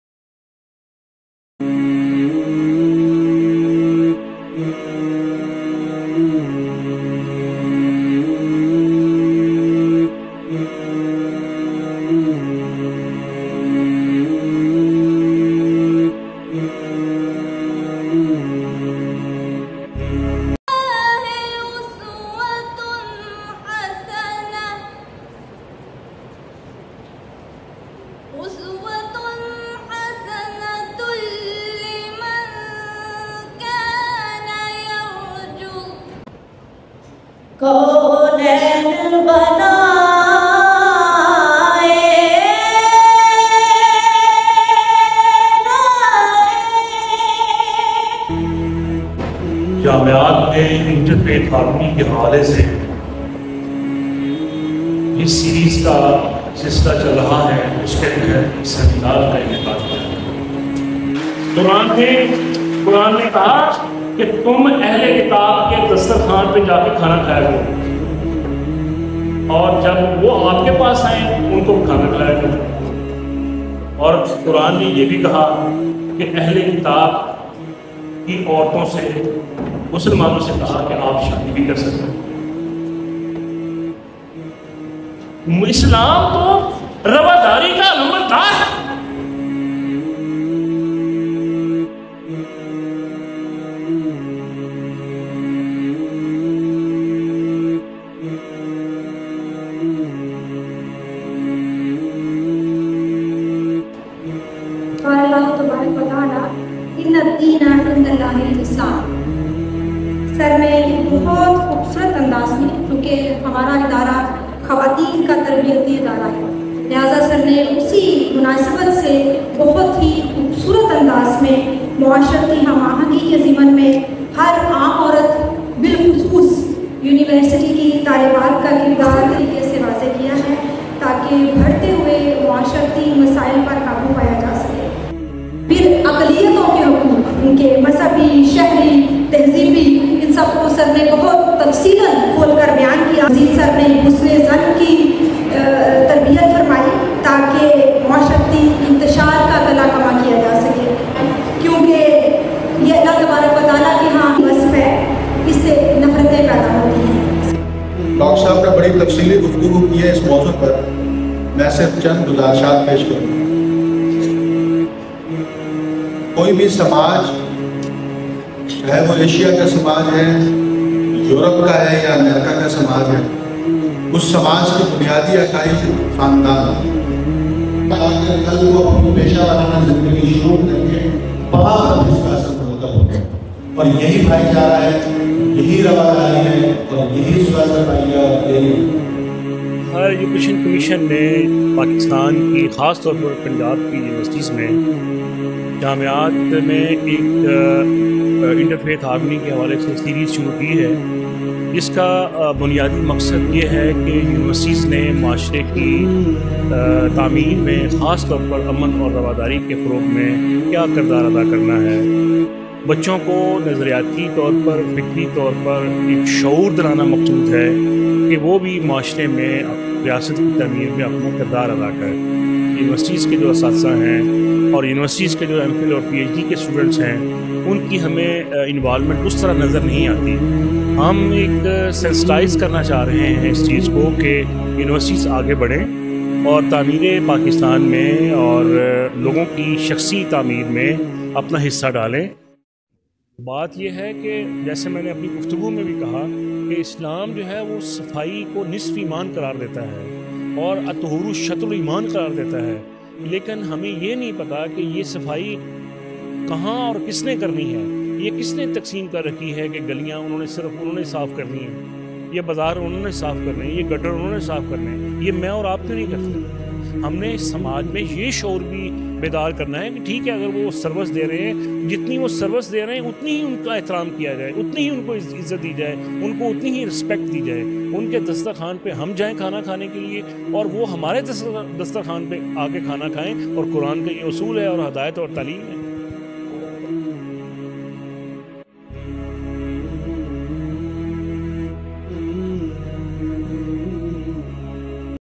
Interfaith Harmony in Pakistan, The Influential Role of University in shaping a Pluralistic Society.
Seminar in Collaboration with Punjab Higher Education Commission and The Government Sadiq College Women University Bahawalpur.
Resource Person of the event was Dr Qasim Tanveer Director Punjab Higher Education Commission.